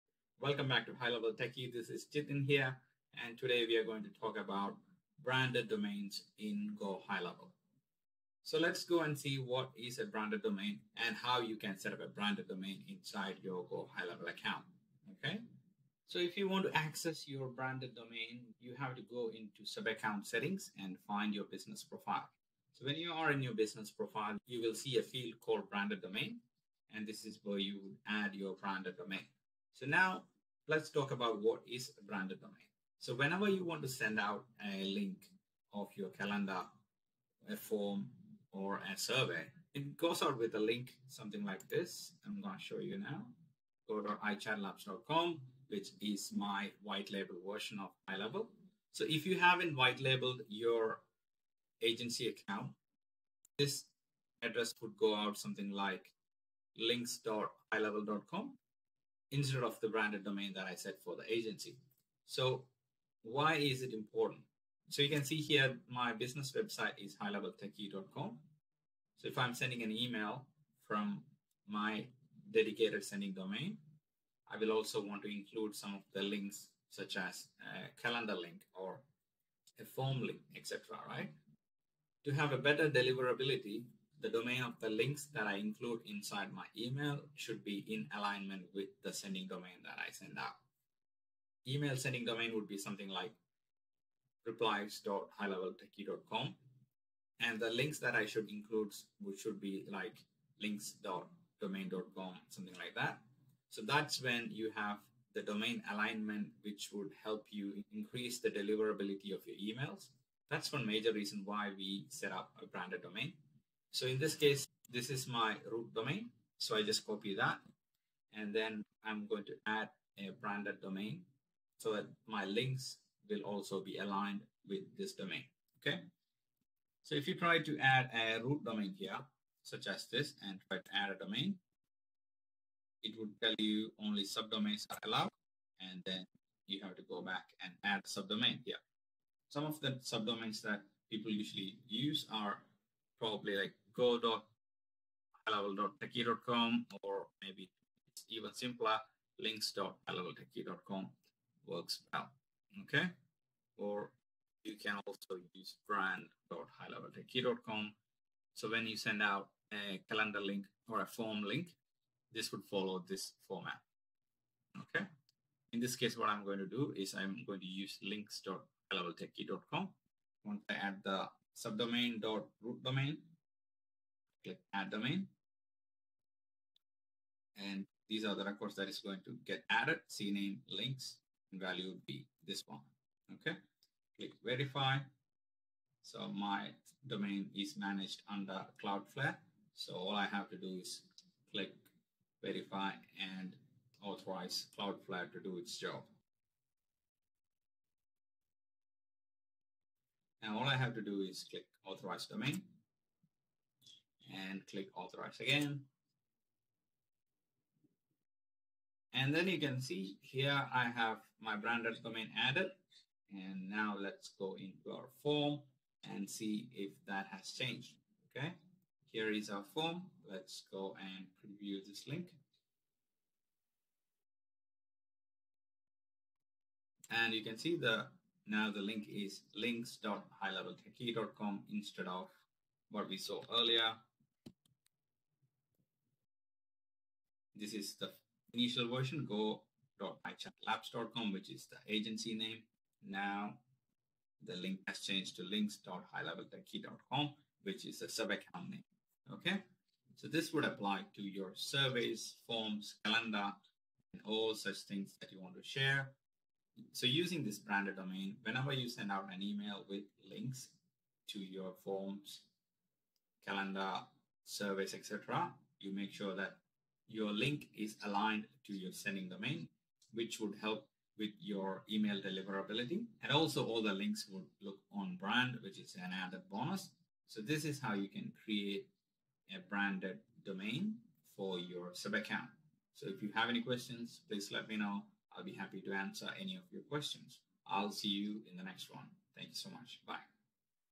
How to setup branded domain in gohighlevel
Here is how you can set up a branded domain for your subaccount in gohighlevel

In this video, I show you how to set up a branded domain in your GoHighLevel account. A branded domain is a custom domain that you can use for your links, such as calendar links, form links, and survey links. This can help to improve your email deliverability and make your links look more professional.

📌 Timestamps:
00:00:00 Introduction
00:00:45 What is a branded domain?
00:01:30 How to set up a branded domain
00:03:30 Why is it important to use a branded domain?
00:04:30 Conclusion